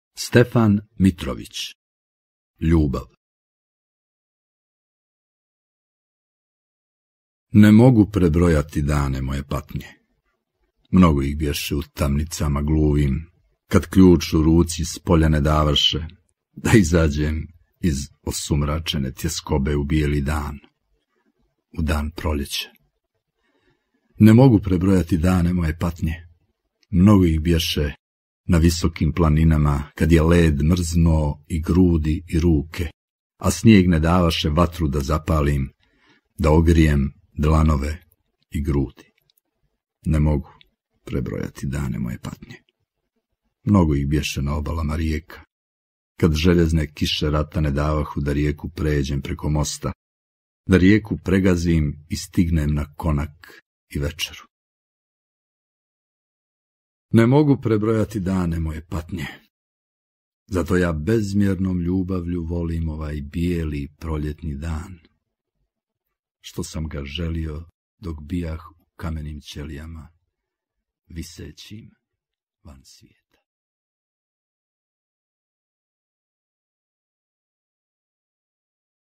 STEFAN MITROVIĆ - LJUBAV
STEFAN MITROVIĆ - LJUBAV  (interpretira Dejan Đonović)

Stefan Mitrović, rođen u Svetom Stefanu 1909. godine. Gimnaziju završio u Kotoru. Studirao na Filozofskom fakultetu u Beogradu. Progonjen i hapšen kao pripadnik revolucionarnog pokreta. Učesnik narodnooslobodilačke borbe od 1941. godine. U toku i poslije rata (do kraja 1948. godine) bio nа visokim partijskim i društvenim funkcijama. Poslije izlaska sa robije živio je u Svetom Stefanu. Umro 1985. godine.

Od 1926. godine objavljivao je svoju poeziju u: zagrebačkoj „Mladosti”; skopskom „Južnom pregledu”; beogradskim časopisima i listovima – „Misao”, „Ilustrovane novine”, „Stožer”, „Linija”, „Smena”, „Razvitak”, „Književne novine”; cetinjskim „Zapisima”; nikšićkim časopisima – „Razvršje” i „Spone”; u novosadskom „Letopisu matice srpske”; u sarajevskom „Pregledu”; titogradskim časopisima i listovima -„Stvaranju”, „Ovdje”, „Pobjeda”.

Djela:

PJESMA I ČOVJEK, pjesme, Cetinje 1971;
SNIJEG I MORE, pjesme, Titograd 1976.